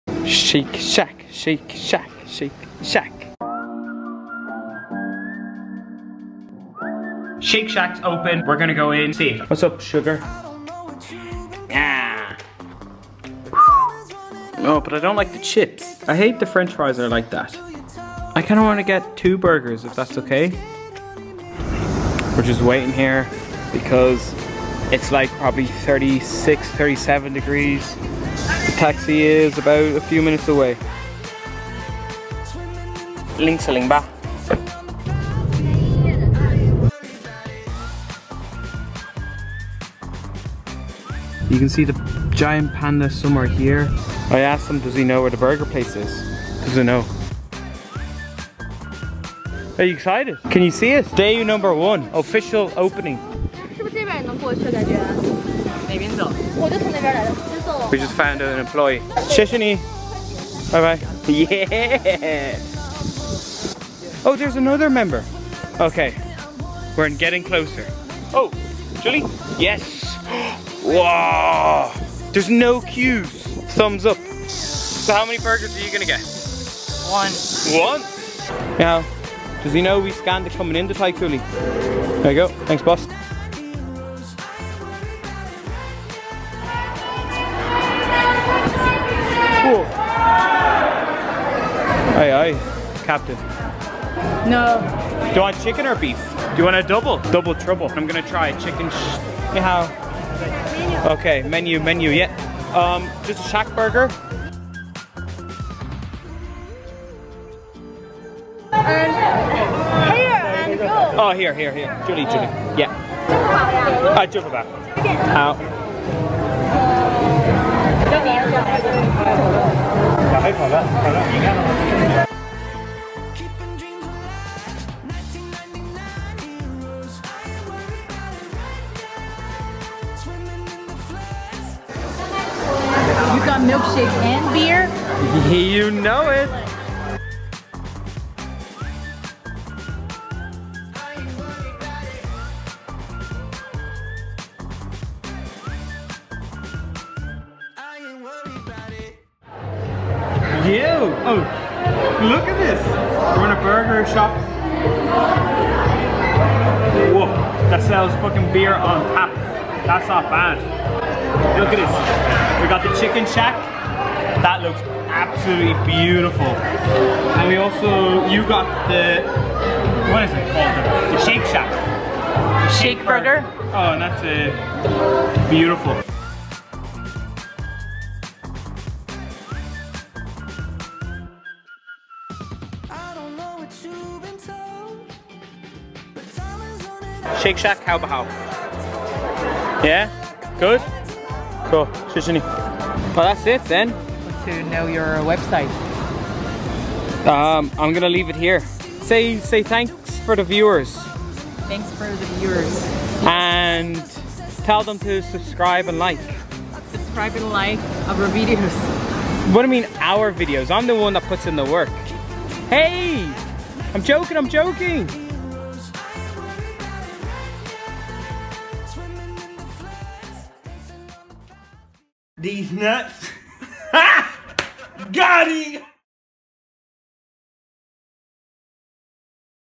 Shake shack just opened up in my city! | China Vlogs
A nice quick video today.
What you guys think?
Im starting to see some Trend that short videos are the thing now.

I sometimes find it hard to put everything in a short vid.

so We went to the grand opening of Shack Shack in Chengdu.
美国（American) burgers are to die for.

Yup Yup
You what to do to make me happy.
Drop a comment, like it, share it.

Socials: harkinkevin94